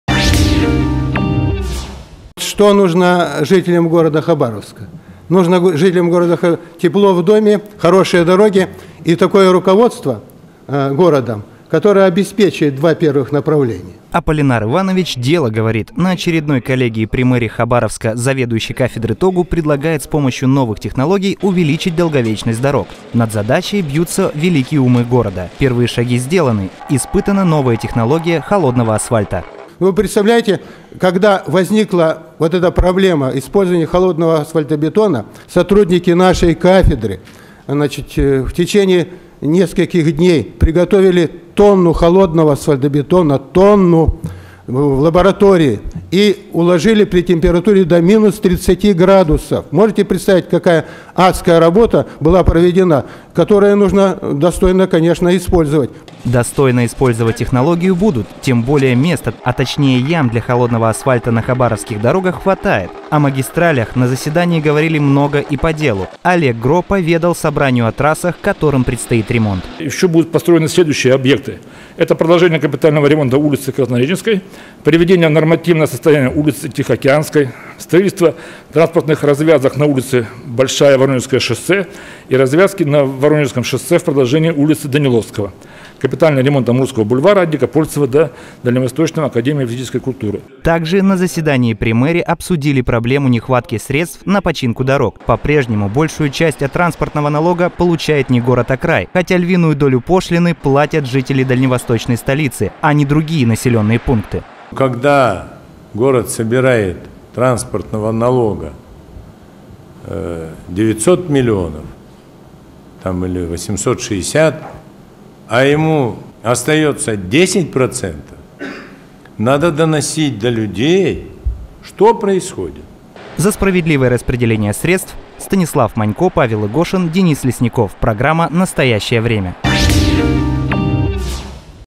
Деньги и дороги
Мы живем в суровой стране! Холода не только нас не пугают, даже асфальт согласен укладываться в минус 30. И это не стёб. Просто технологии позволяют. О новых методиках, нехватке денег и дорожных планах — в сюжете.